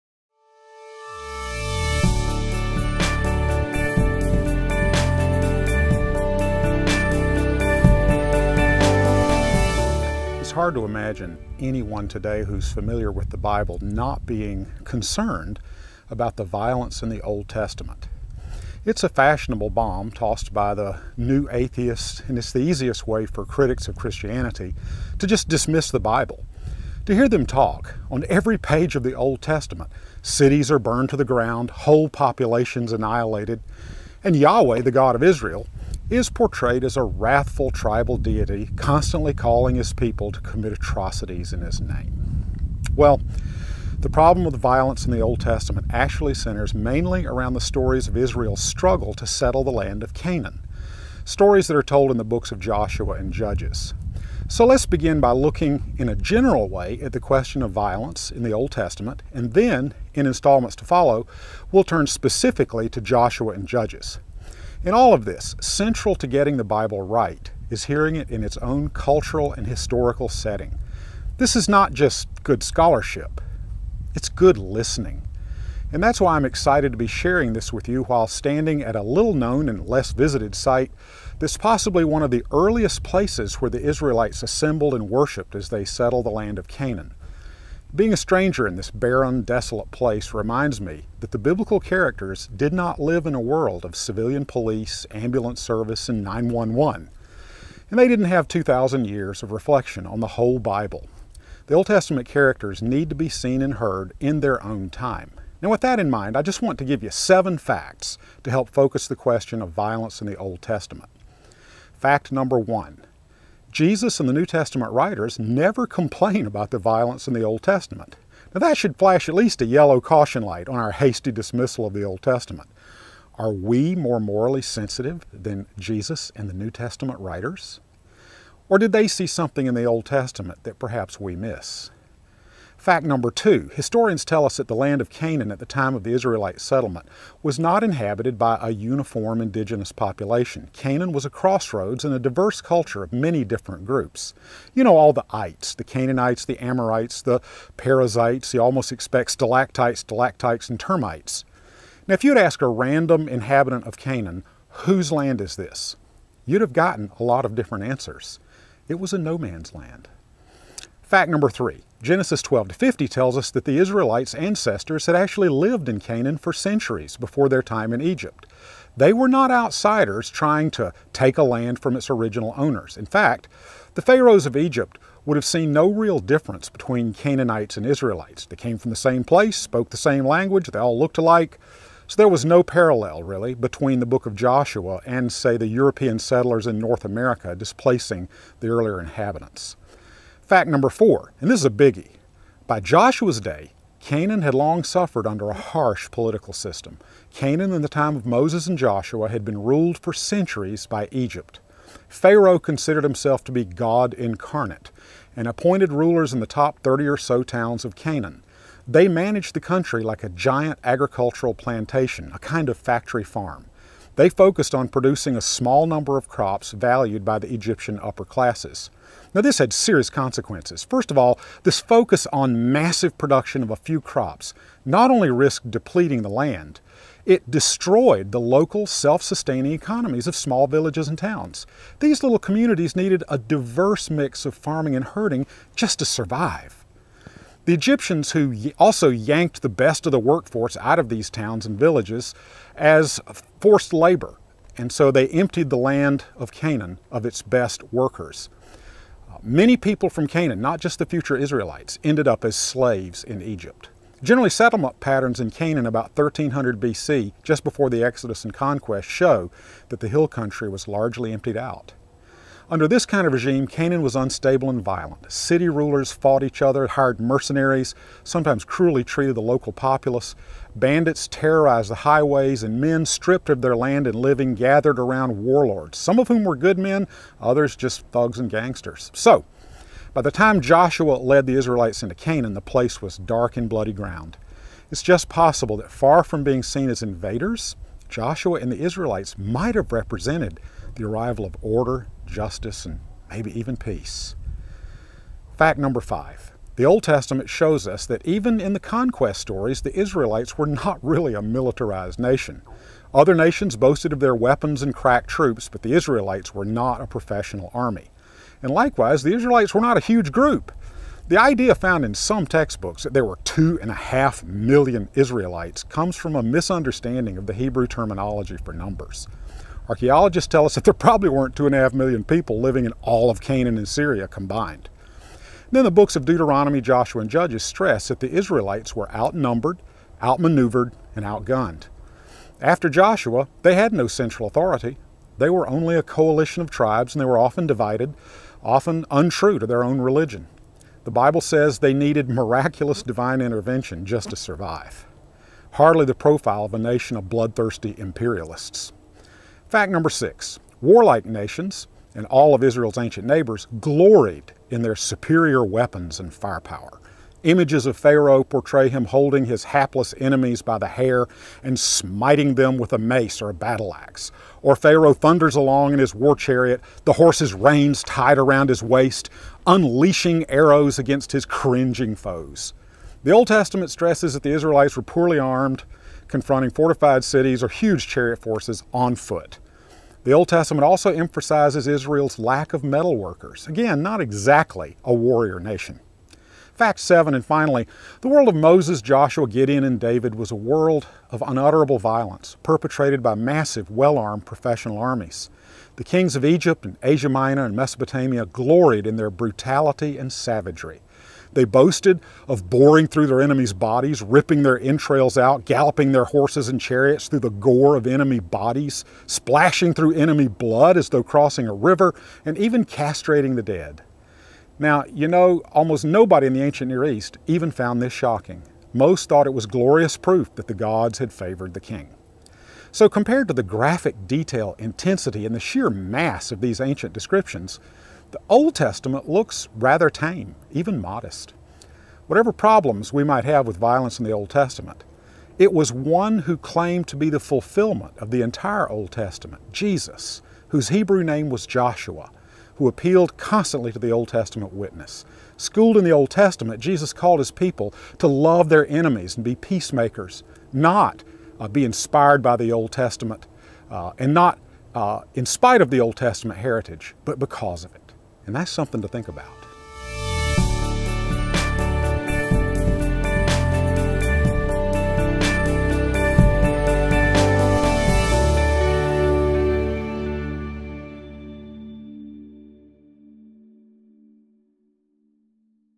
Violence in the Old Testament (Part 1) Lawson Stone
Dr. Lawson Stone shares seven points we must keep in mind when encountering violence in the Old Testament.